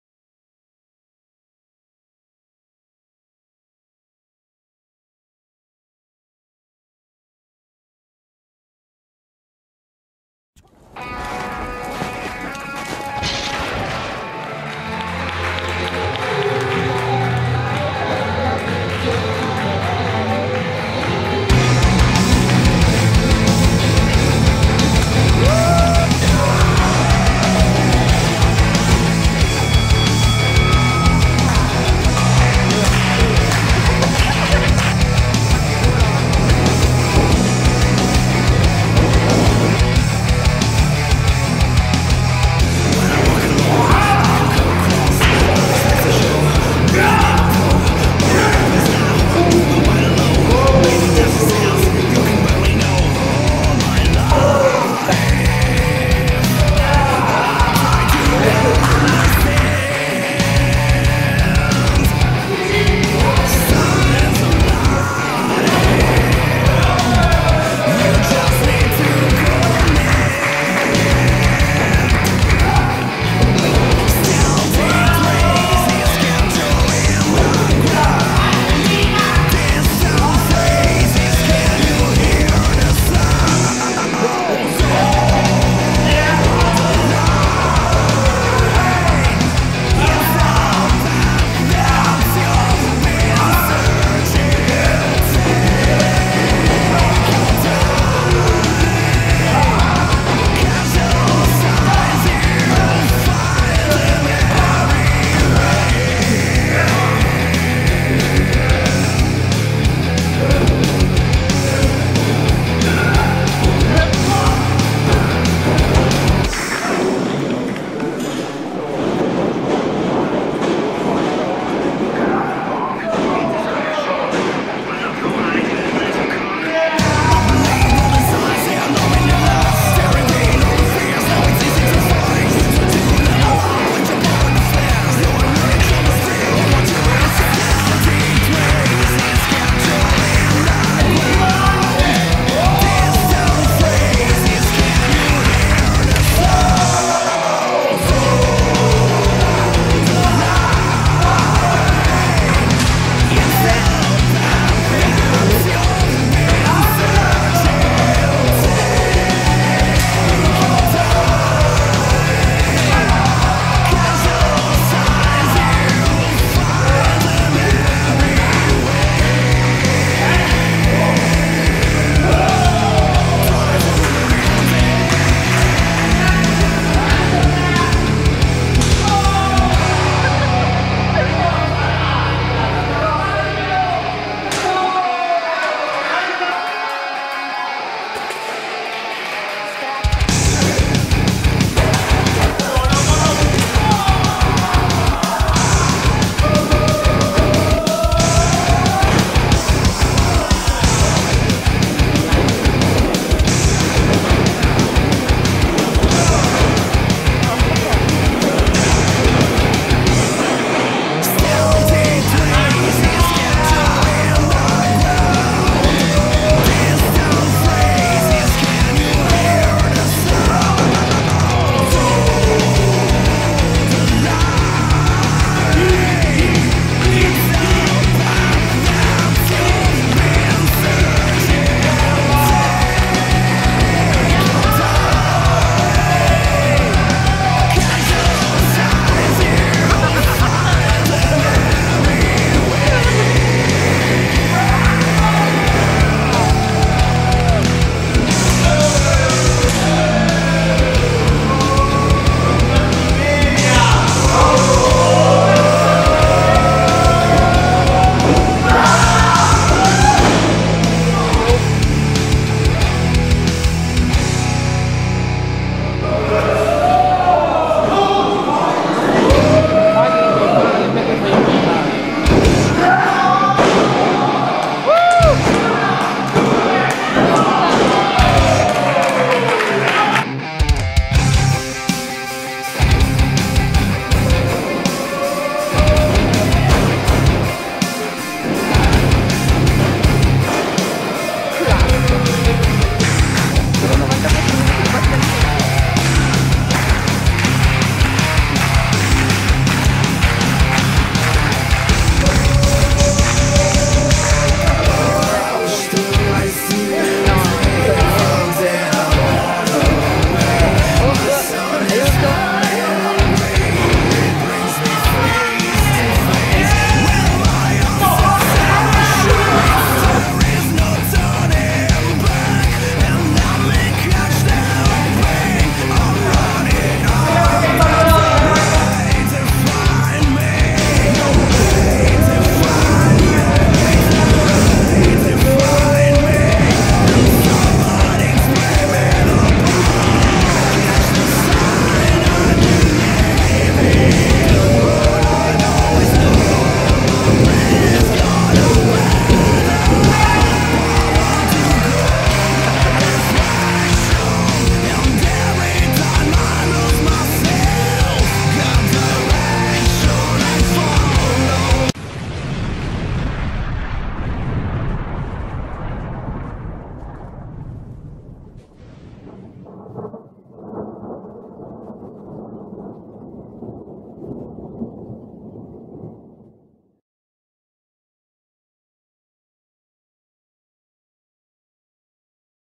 MAW Eye of the Storm HIGHLIGHT: Marcio Silva vs The Greatest (Champ) per il titolo MAW InsurreXtion
Marcio Silva si aggiusica la possibilà di andare per il titolo InsurreXtion, tenuto da The Greatest, vincendo il 3 Way Dance ad inizio show.